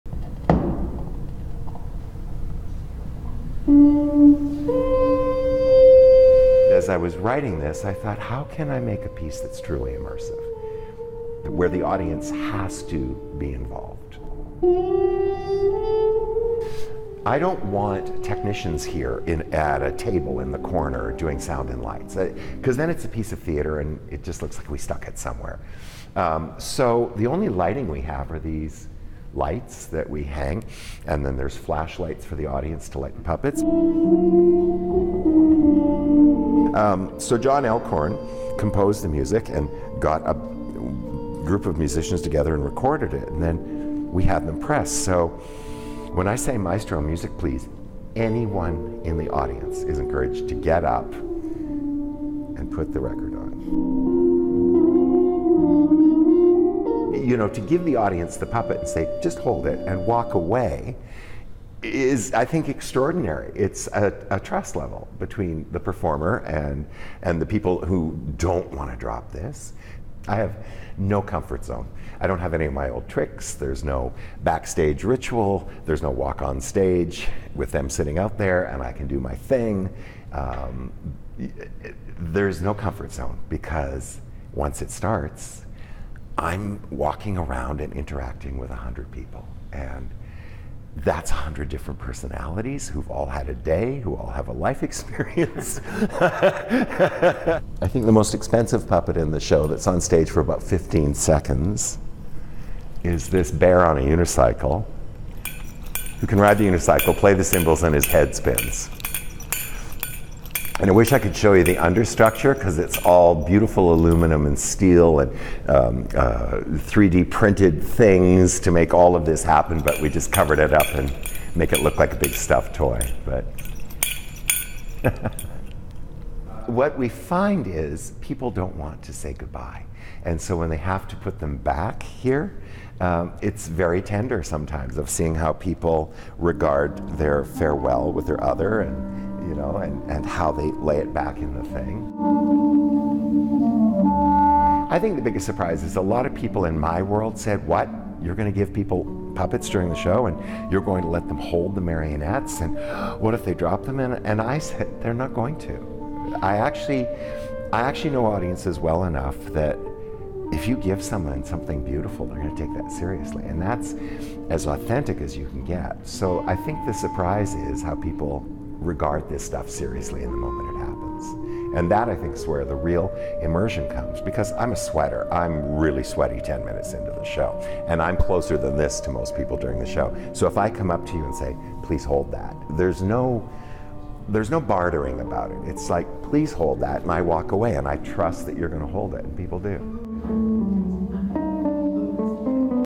Puppet master crafts theatre show that audience can join
Canadian puppet master Ronnie Burkett's new show 'Forget Me Not' is a theatrical love letter and one-of-a-kind experience. The Order of Canada recipient is in Vancouver with his troupe of handmade marionettes for the next month and spoke to CBC News about the creative inspiration behind his latest show.